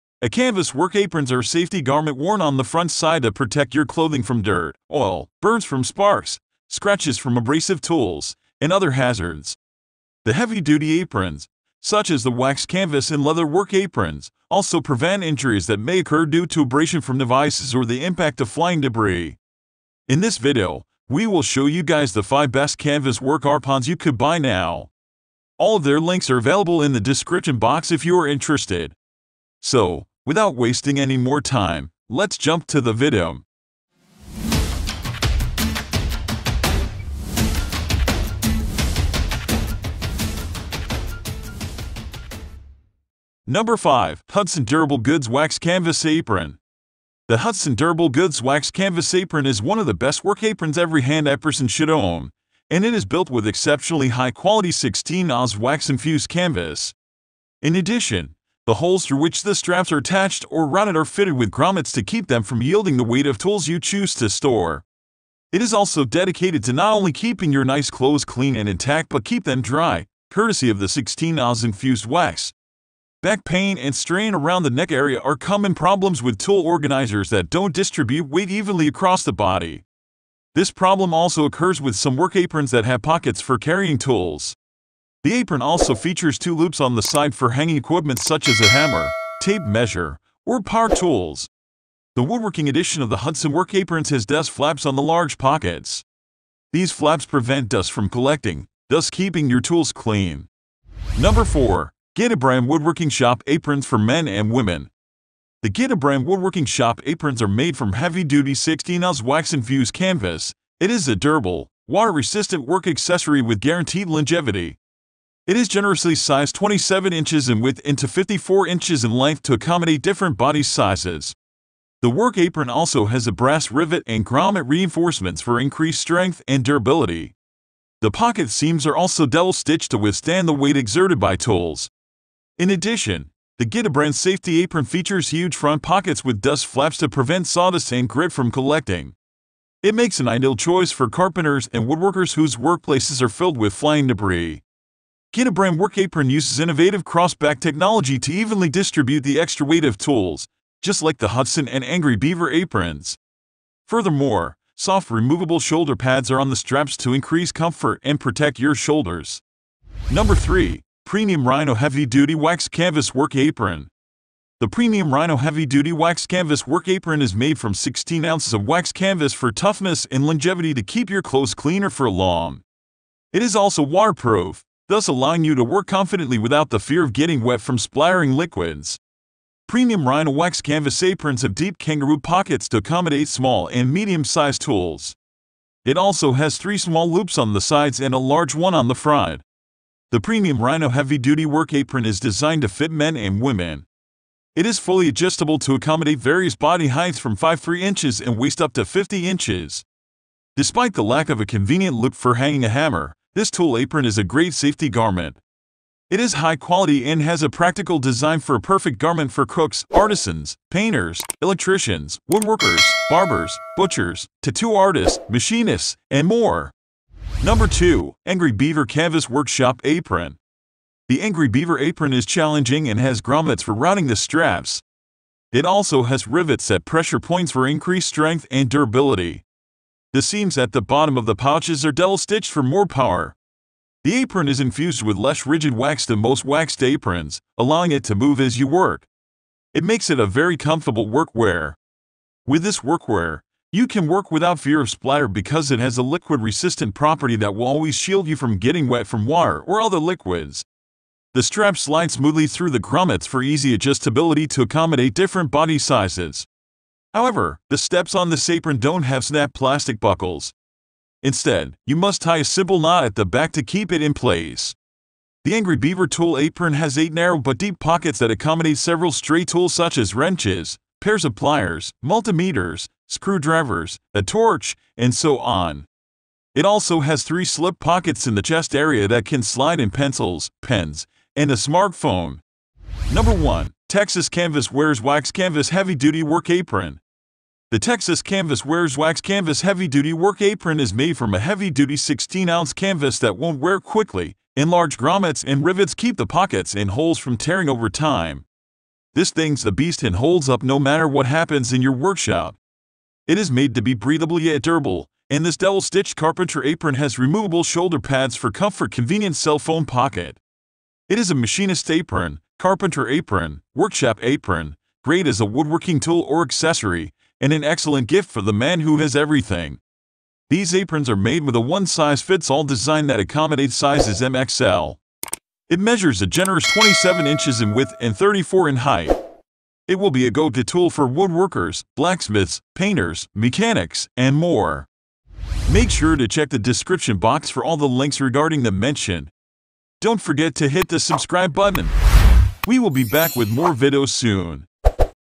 Top 5 Best Canvas Work Aprons 2023
A canvas work aprons are a safety garment worn on the front side to protect your clothing from dirt, oil, burns from sparks, scratches from abrasive tools, and other hazards. The heavy-duty aprons, such as the waxed canvas and leather work aprons, also prevent injuries that may occur due to abrasion from devices or the impact of flying debris.

In today’s video, we got you a review of 5 Best Canvas Work Aprons you could buy in 2023.

➜ 5. Hudson Durable Goods Waxed Canvas Apron

➜ 4. GIDABRAND Woodworking Shop Aprons for Men and Women

➜ 3. Premium Rhino Heavy Duty Waxed Canvas Work Apron

➜ 2. Angry Beaver Canvas Work Shop Apron

➜ 1. Texas Canvas Wares Waxed Canvas Heavy Duty Work Apron